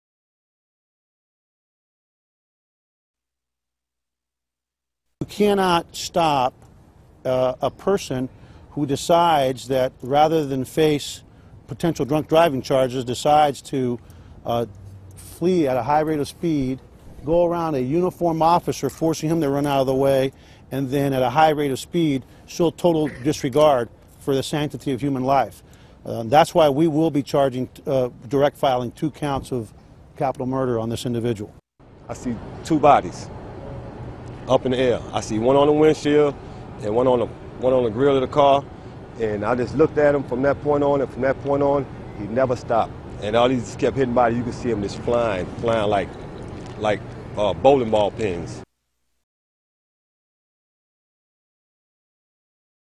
Drunk driver faces capital murder charges for SXSW crash
Austin Police Chief Art Acevedo reveals the driver of a car which crashed into a crowd at the music festival in Texas will face charges over the deaths of two people.